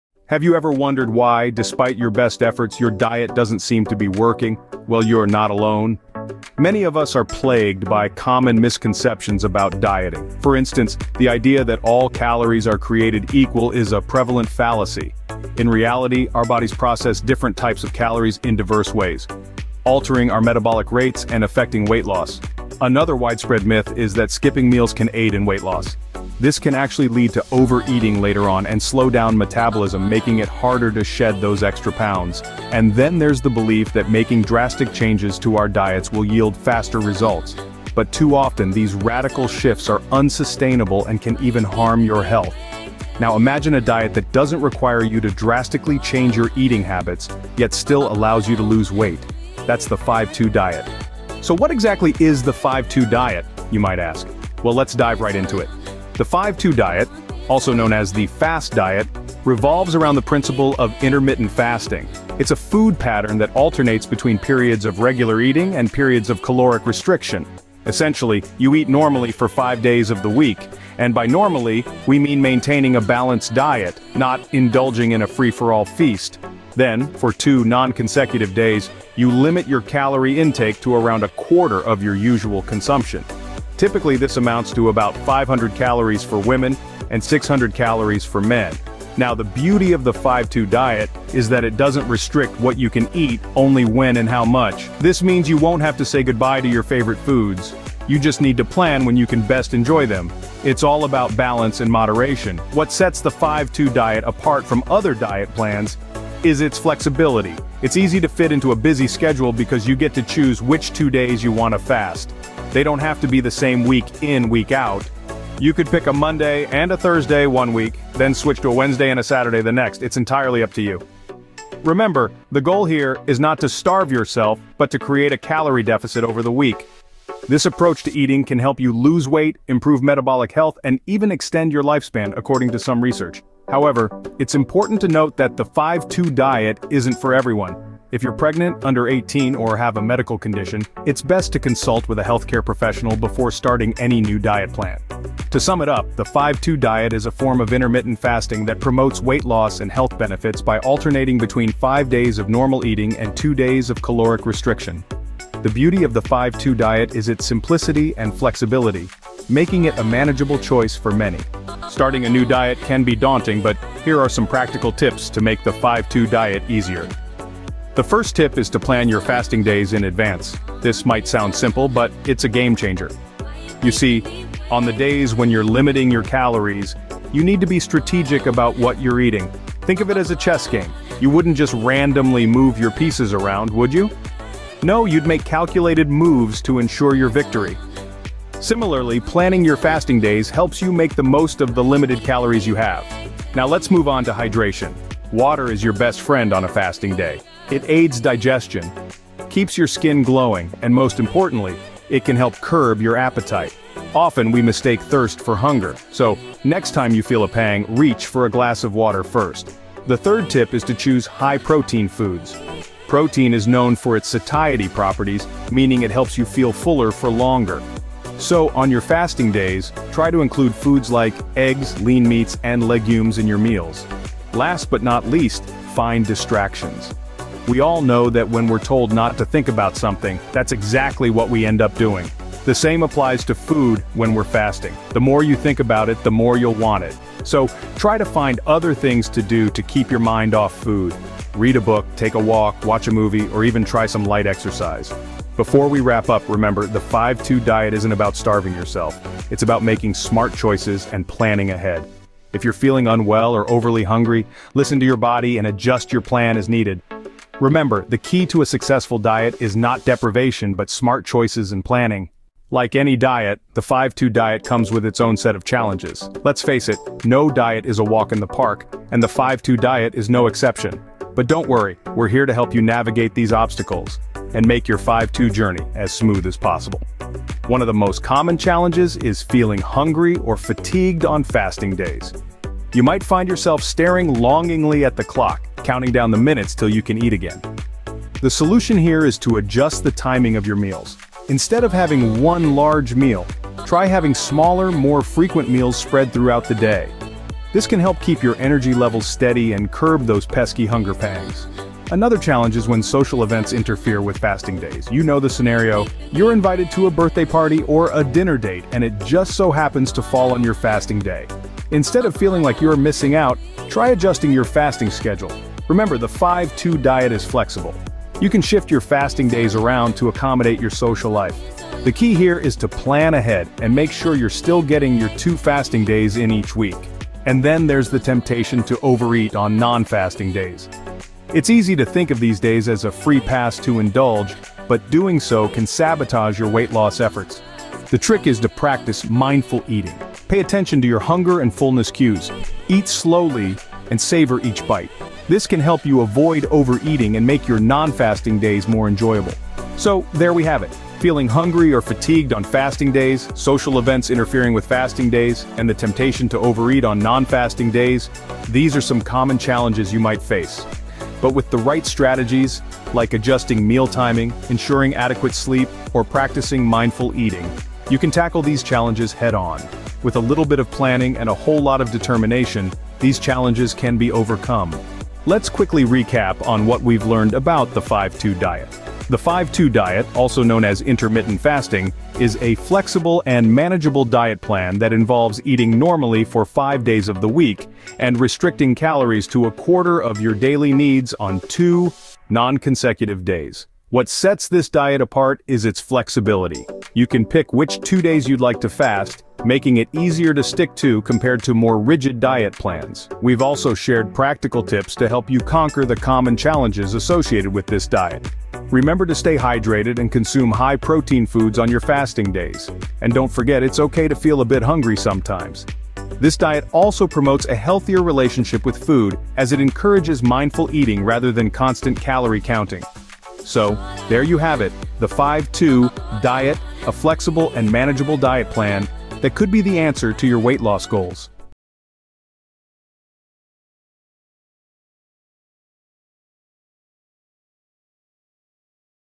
5:2 Diet(Intermittent Fasting) with these Essential Tips!
5:2 Diet(Intermittent Fasting) with these Essential Tips! Unlock the secrets to mastering the 5:2 diet with these essential tips and tricks! In this video, Health&Weight World walks you through the basics of intermittent fasting, focusing on how to effectively implement the 5:2 diet into your lifestyle.

 From meal planning and portion control to staying motivated and overcoming challenges, Health&Weight World has your back. Whether you're new to fasting or want to improve your current routine, these practical strategies will help you succeed. Don't miss out on achieving your health goals – watch now!

OUTLINE:
00:00:00 Debunking Dieting Misconceptions
00:00:51 5:2 Diet Explained
00:02:41 5:2 Diet Tips
00:04:22 Common Challenges and Solutions
00:06:15 5:2 Diet Recap and Conclusion